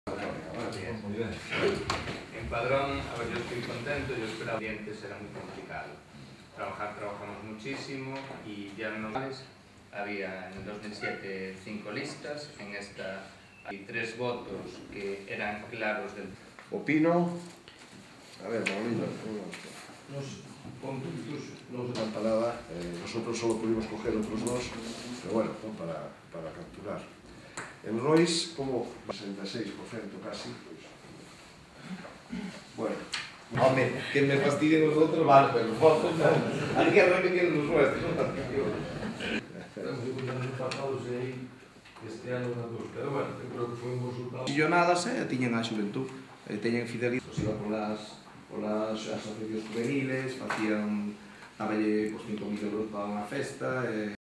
Reuni'on de Carlos Negreira cos alcaldes e portavoces do PP do Partido Xudicial de Santiago
o presidente provincial do PP na Coruña, Carlos Negreira, reúnese cos alcaldes e portavoces do PP do Partido Xudicial de Santiago para avaliar os resultados do pasado 22 de maio